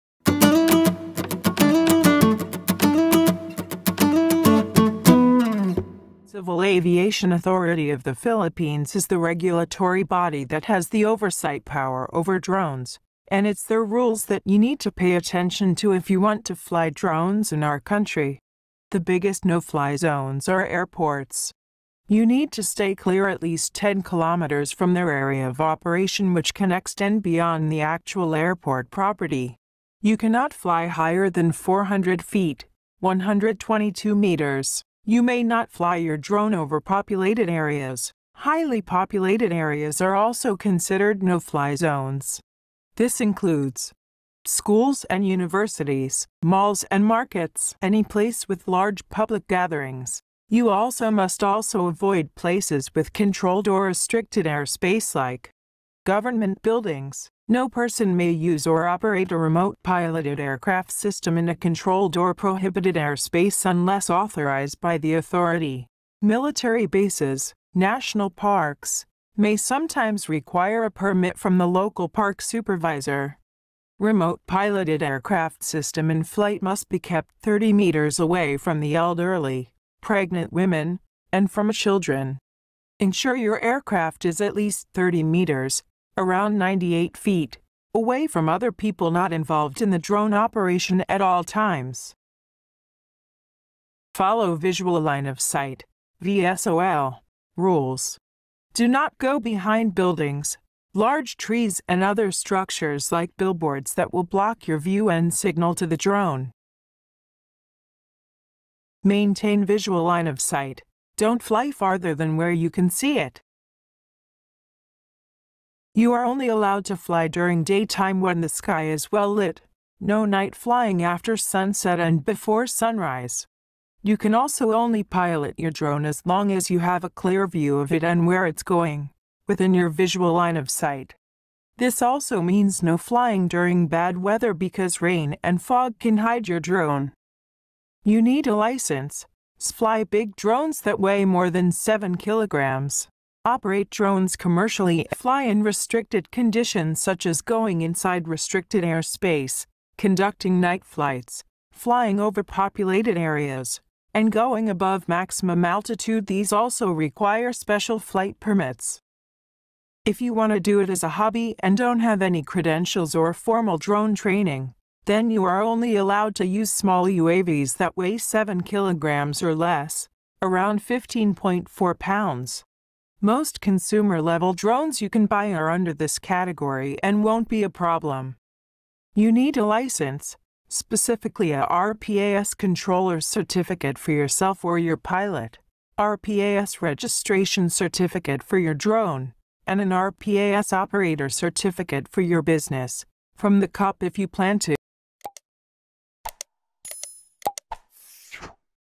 Drone Rules and Regulations in the Philippines, CAAP Restrictions
Drone flying is fun but we should know our limitations. This videos enumerates the different rules and regulations under Civil Aviation Authority CAAP  in the Philippines. We may face serious fines and consequences if we do not adhere with these restrictions.